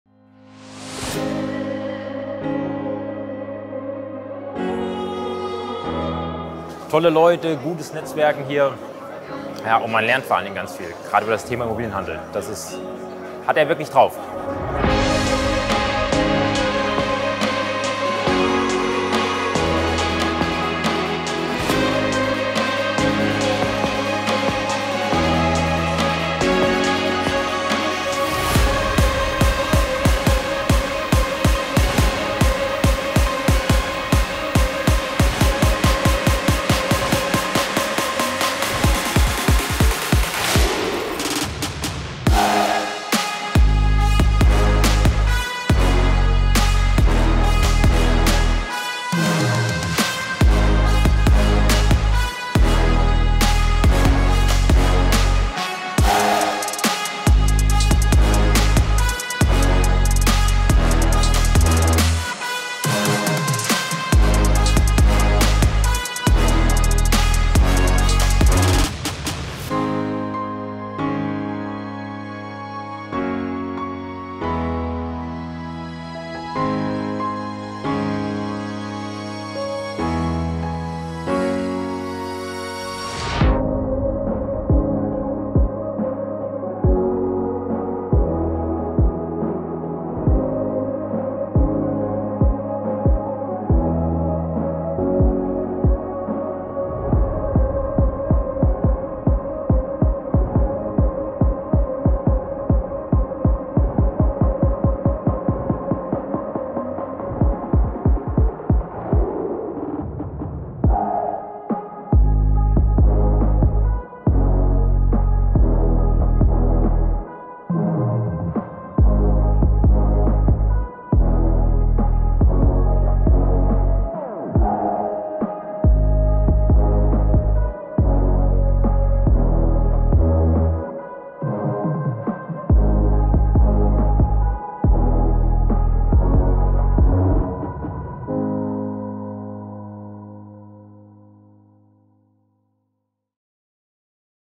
FIX&FLIP Live Event - Oliver Fischer Immobilienhandel
Immobilien kaufen verkaufen...

FIX und FLIP - Wie Du mit einfachen Immobilien kurzfristige Gewinne erzielst, ohne aufwändige Sanierung und Mieterstress.
Starte nebenbei in Deiner Nachbarschaft mit dem  FIX&FLIP-Immobilienhandel (gewerblicher Grundstückshandel).
Das ist ein kurzfristiges Immobilieninvestment, was viele Immobilien-Investoren zum Aufbau von Eigenkapital nutzen.